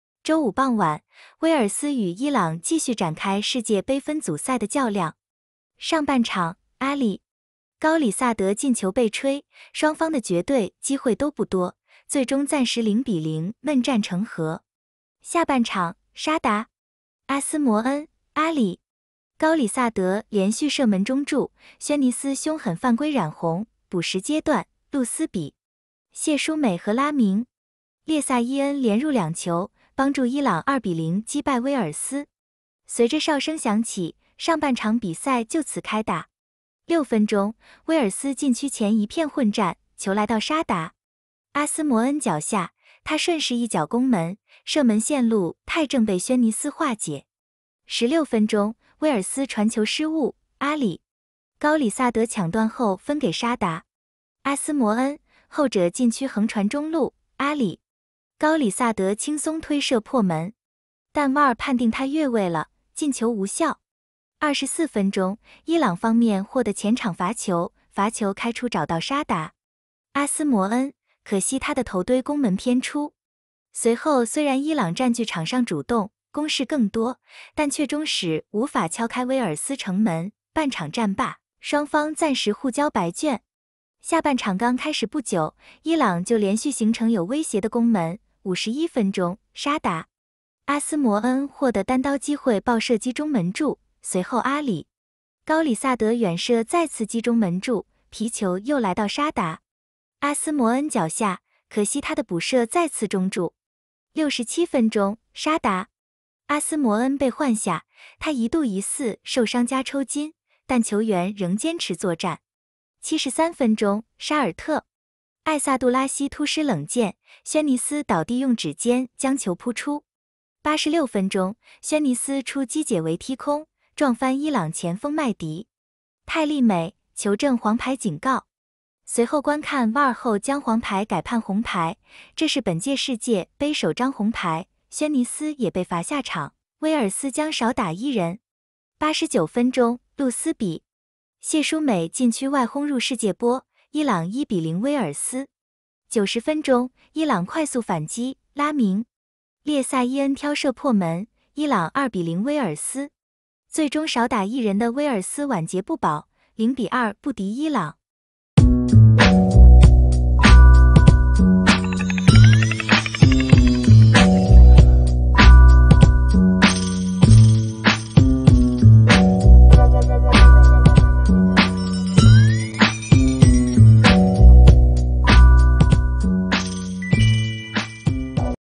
(普)【世界盃-賽後新聞】2022-11-25 威爾斯 0:2 伊朗 | 伊朗2球擊敗威爾斯
週五傍晚，威爾斯 與#伊朗 繼續展開#世界盃 分組賽的較量。上半場，阿里.高里薩德進球被吹，雙方的絕對機會都不多，最終暫時0比0悶戰成和。下半場，沙達.阿斯摩恩、阿里.高里薩德連續射門中柱，軒尼斯兇狠犯規染紅，補時階段，路斯比.謝舒美和拉明.列薩伊恩連入2球，幫助伊朗2比0擊敗威爾斯。

隨著哨聲響起，上半場比賽就此開打。6分鐘，威爾斯禁區前一片混戰，球來到沙達.阿斯摩恩腳下，他順勢一腳攻門，射門線路太正被軒尼斯化解。16分鐘，威爾斯傳球失誤，阿里.高里薩德搶斷後分給沙達.阿斯摩恩，後者禁區橫傳中路，阿里.高里薩德輕鬆推射破門！但VAR判定他越位了，進球無效！24分鐘，伊朗方面獲得前場罰球，罰球開出找到沙達.阿斯摩恩，可惜他的頭鎚攻門偏出。隨後雖然伊朗佔據場上主動，攻勢更多，但卻終始無法敲開威爾斯城門，半場戰罷，雙方暫時互交白卷。

下半場剛開始不久，伊朗就連續形成有威脅的攻門，51分鐘，沙達.阿斯摩恩獲得單刀機會爆射擊中門柱，隨後阿里.高里薩德遠射再次擊中門柱，皮球又來到沙達.阿斯摩恩腳下，可惜他的補射再次中柱。67分鐘，沙達.阿斯摩恩被換下，他一度疑似受傷+抽筋，但球員仍堅持作戰。73分鐘，沙爾特.艾薩杜拉希突施冷箭，軒尼斯倒地用指尖將球撲出。86分鐘，軒尼斯出擊解圍踢空，撞翻伊朗前鋒麥迪.泰利美，球證黃牌警告。隨後觀看VAR後將黃牌改判紅牌，這是本屆世界盃首張紅牌，軒尼斯也被罰下場，威爾斯將少打一人。89分鐘，路斯比.謝舒美禁區外轟入世界波，伊朗1比0威爾斯。90分鐘，伊朗快速反擊，拉明.列薩伊恩挑射破門，伊朗2比0威爾斯。最終少打一人的威爾斯晚節不保，0比2不敵伊朗。